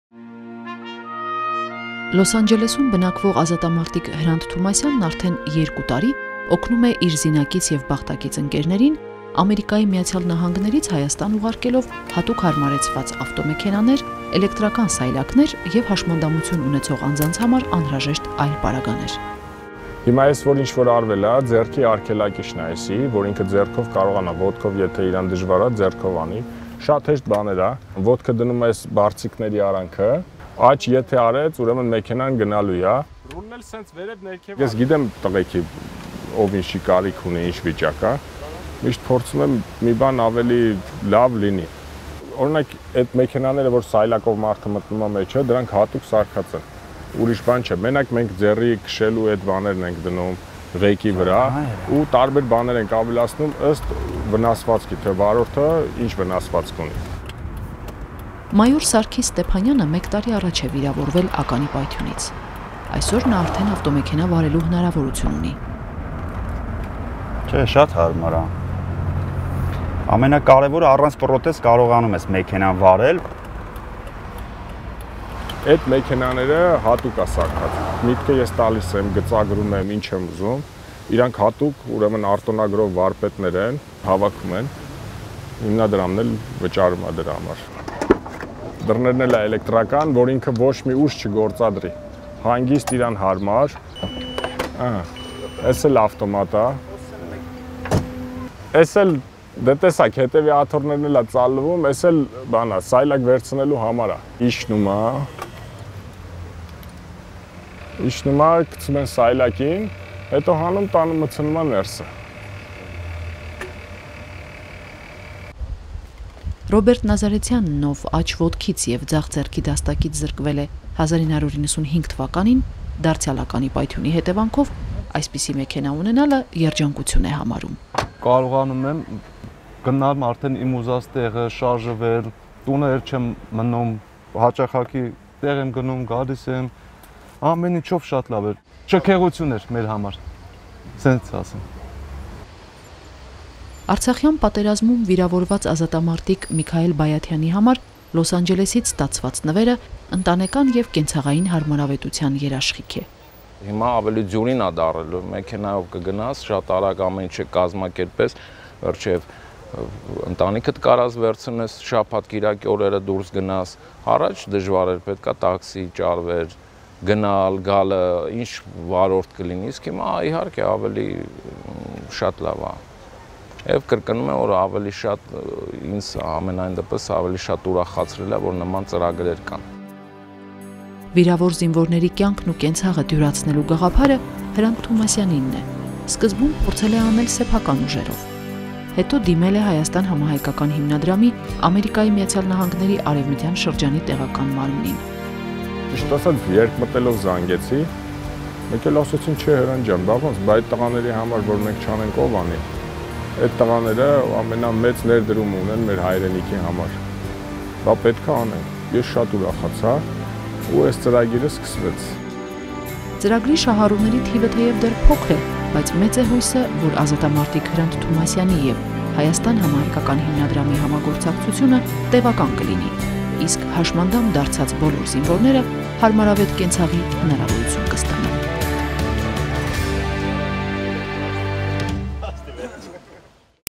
Supporting Wounded Karabakh Veterans
This 2015 video features the work of Hrant Tumasyan of Los Angeles, a veteran of the Karabakh War, who, since 2013, has been helping his fellow disabled veterans in Armenia by providing them with modified cars, electric wheelchairs, and other items needed by persons with physical impairments.